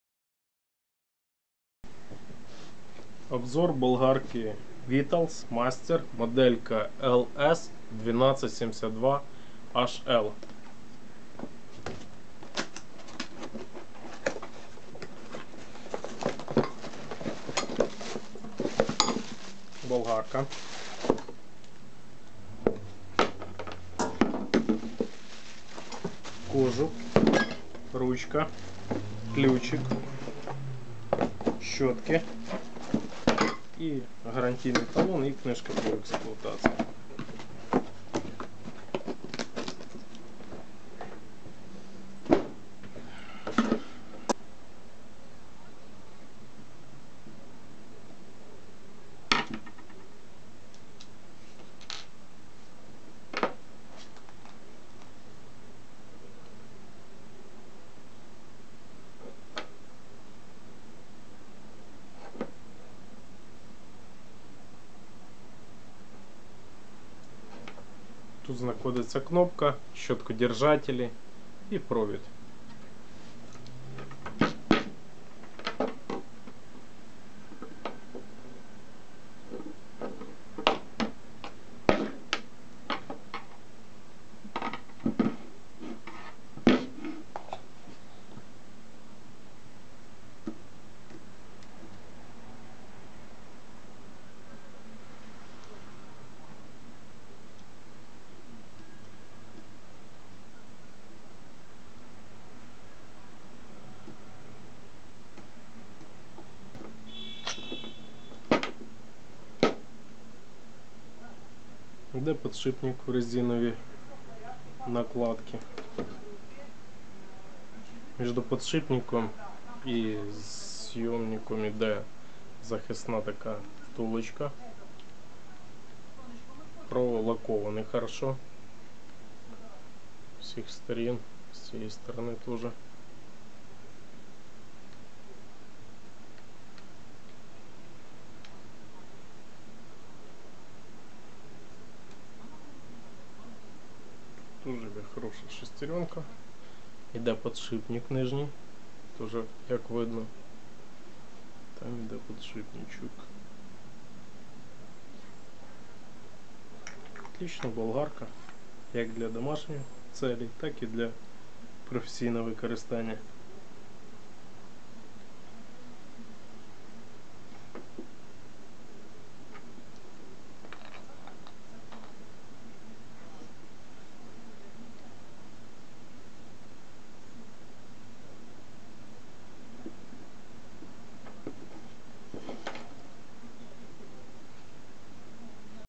Обзор з Болгарки VITALS Ls 1272HL Что внутри?
Розбор болгарки 125 мм VITALS Ls 1272HL Что внутри?
Болгарка VITALS Master Ls 1272HL